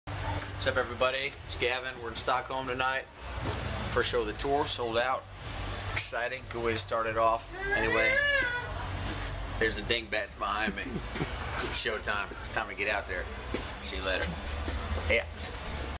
Gavin DeGraw in Stockholm, SE
Gavin DeGraw right before heading on stage in Stockholm, SE. for tour dates and VIP packages!